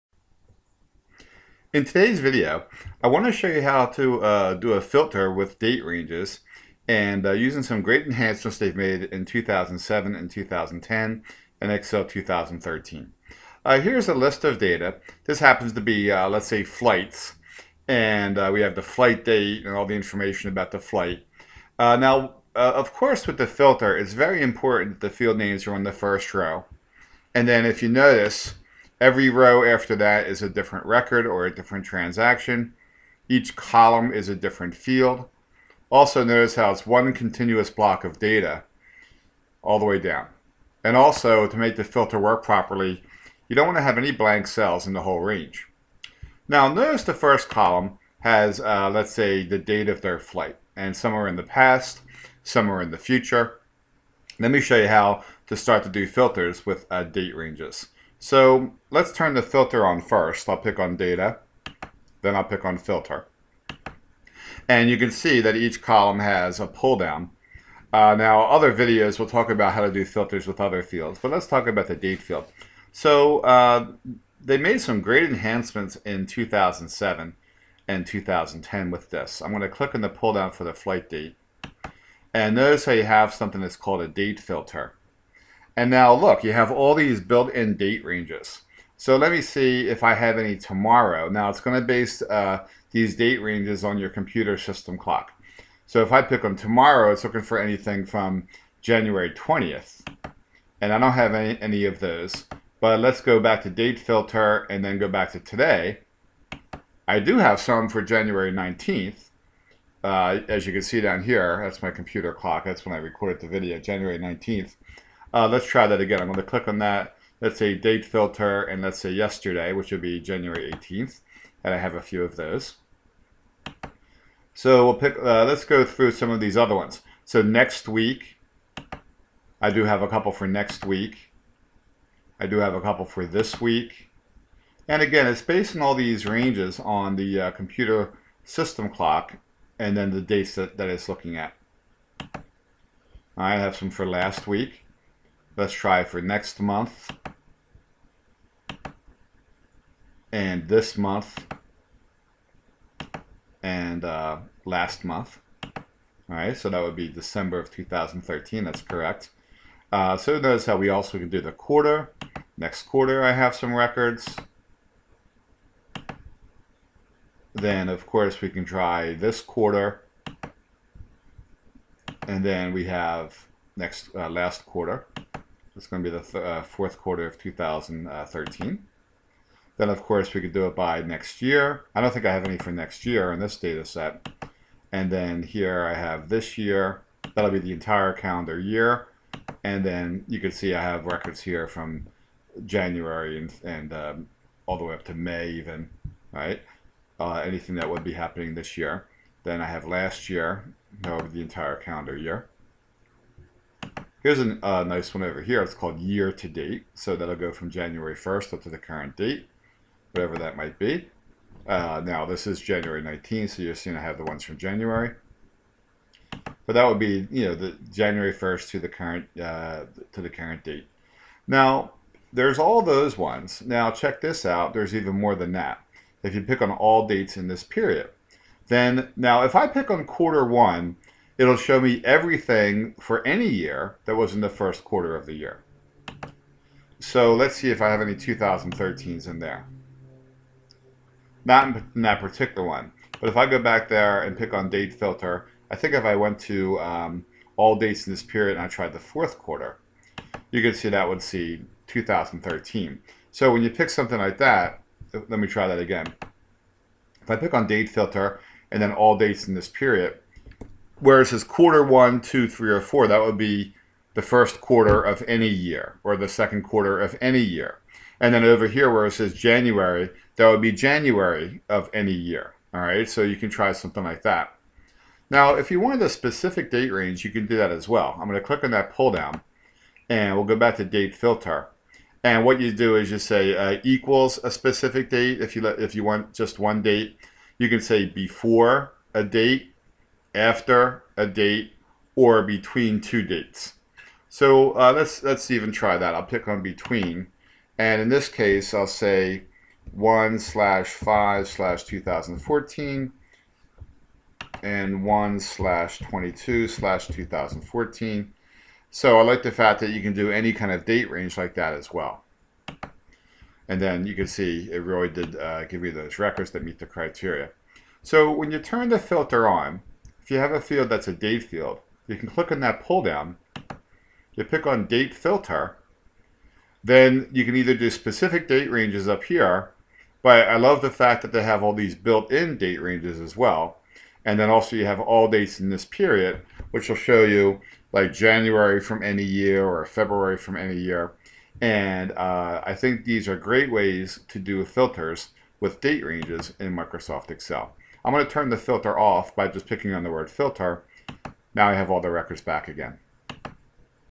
How to apply Excel date Filters to find a certain date range in your Excel data 2010 2013 2016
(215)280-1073

This video will show you how to apply Excel date Filters to find a certain date range in your Excel data.